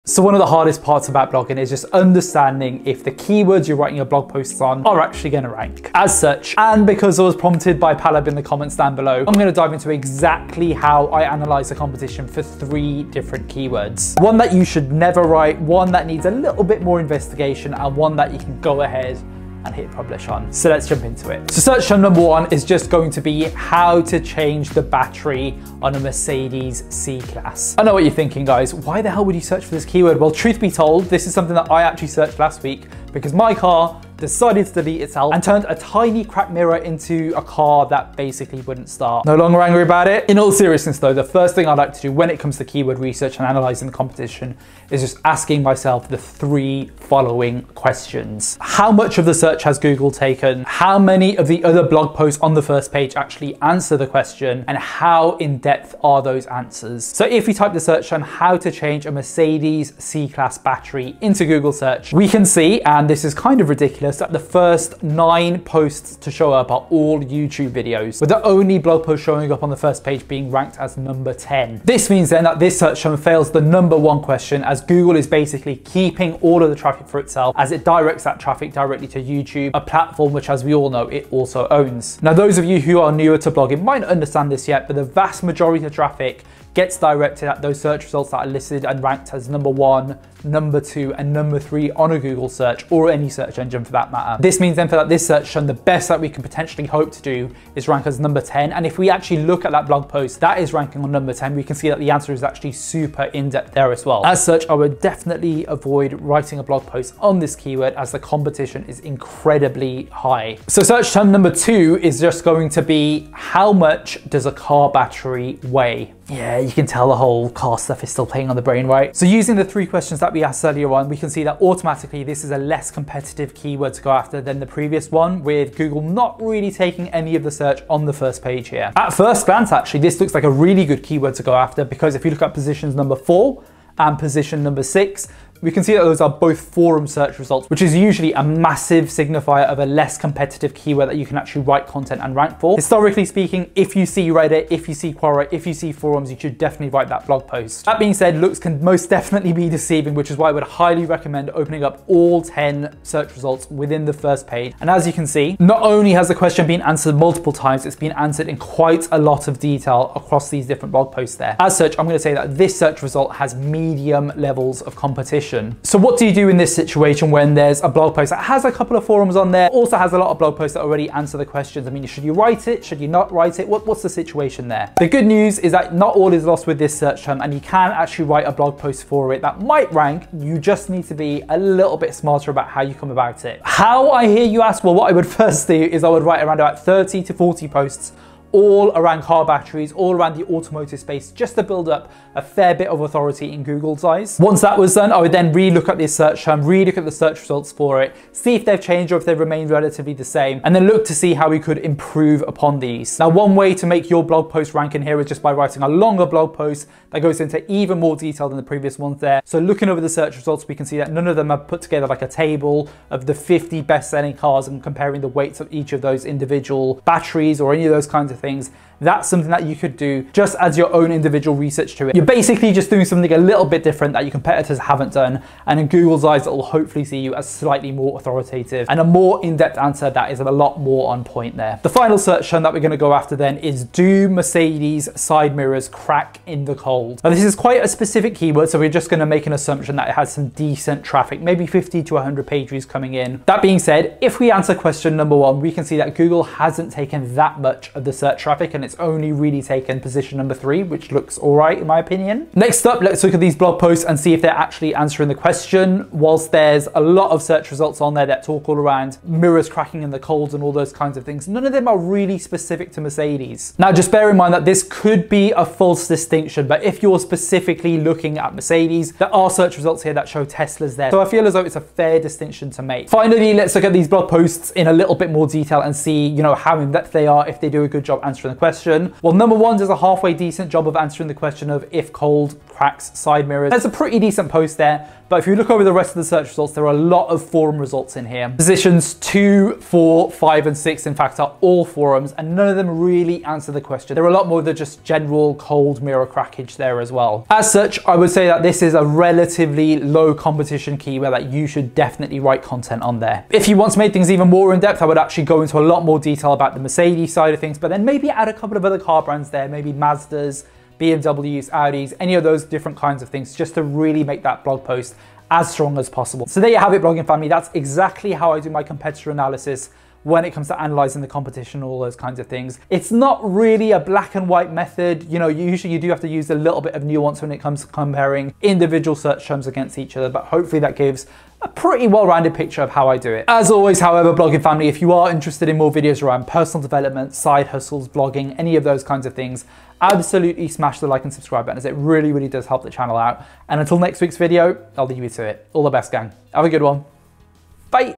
How To Analyse Keyword Competition - Free SEO training, don't get beaten and outrank competition
RECOMMENDED COURSE (Project 24) (link takes you to the Income school website)

Analyzing keywords can be hard, especially when there are tons of similar blog posts all going after the exact same keywords.

So what exactly is a high-competition keyword that you should avoid compared to a low-competition keyword that you can write a blog post on?

Especially if you are a beginner?

B-roll: Pexels.com